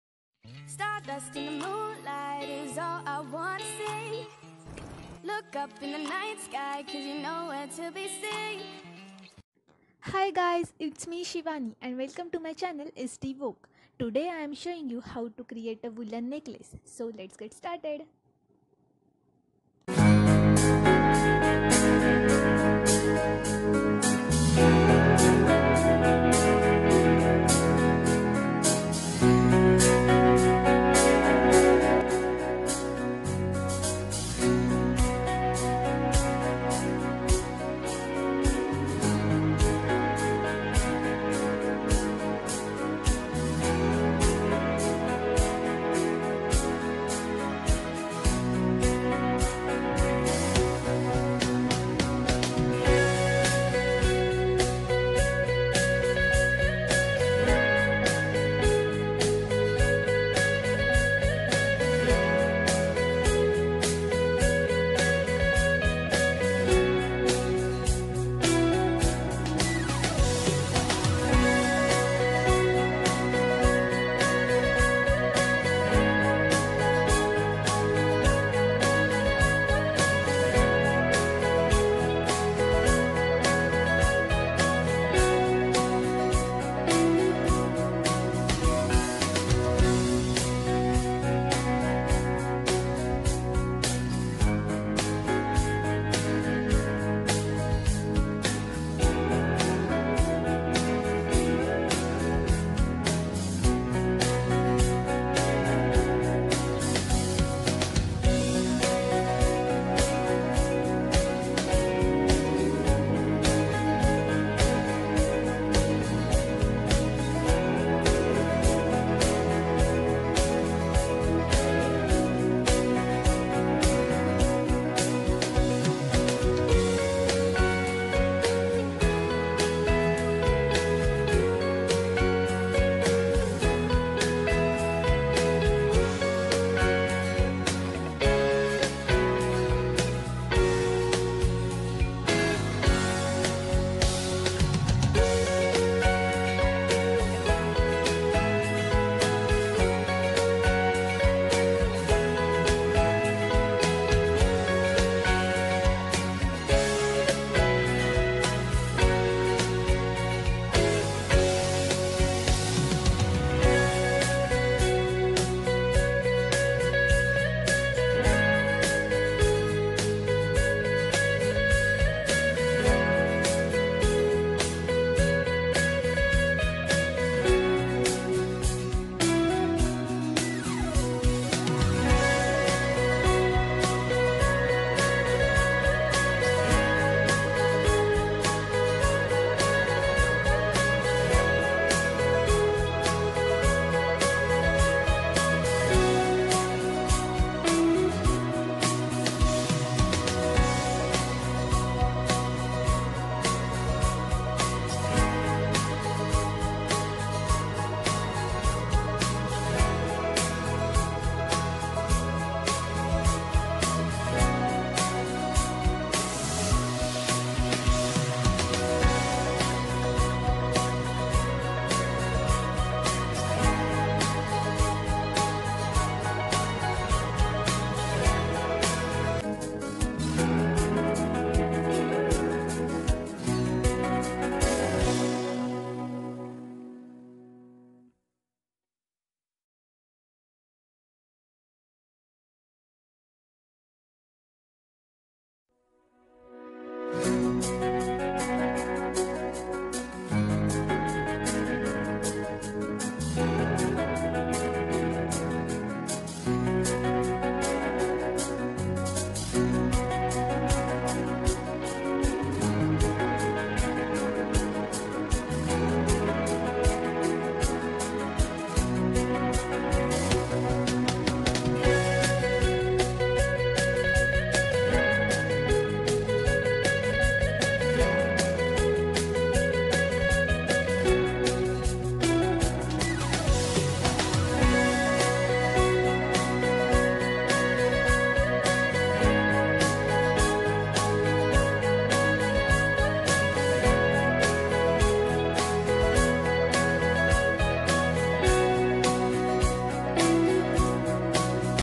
How to make Woolen Necklace | Handmade Trendy Necklace At Home | SD Vogue
So guys this is my first video on YouTube. I hope you will find this interesting. Today's video is about Handmade Woolen Necklace.